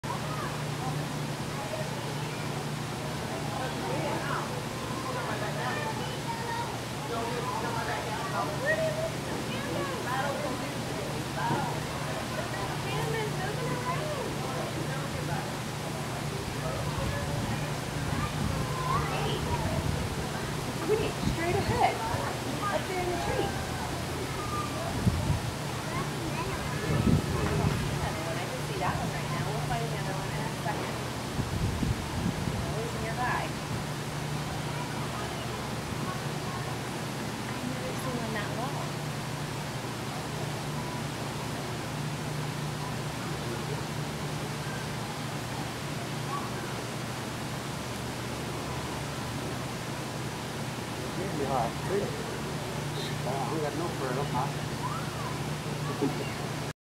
Tired Red Panda
I wonder if he is wondering where Lucille ran off to, or if he is just happy for the quiet?

Mars {male}
Memphis Zoo
Memphis, Tennessee